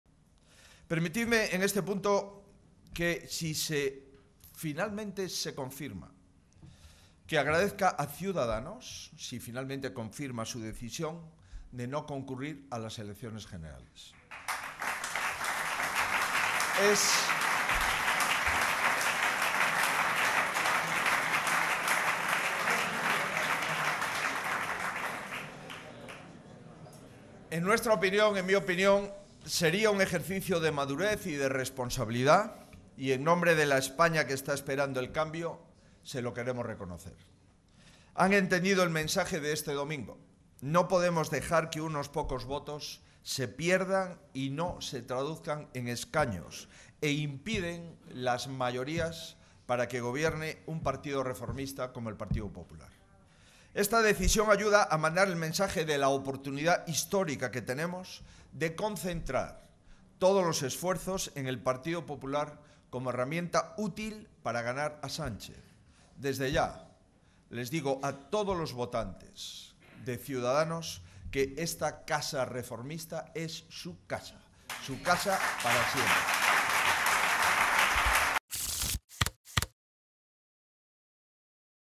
Feijóo agradece a CS no presentarse: "Esta es su casa reformista y no se puede perder un solo voto"
"El sanchismo acabará en 54 días". Así ha concluido Alberto Núñez Feijóo su discurso ante la Junta Directiva Nacional del PP en la que ha valorado los resultados de las elecciones autonómicas y municipales y el anuncio de adelanto de las generales realizado ayer por Pedro Sánchez. El líder popular también ha valorado positivamente la decisión de Ciudadanos, adelantada por Vozpópuli, de no presentarse a esos comicios y ha abierto la puerta del partido a sus votantes: "Esta es su casa reformista y no se puede perder un solo voto".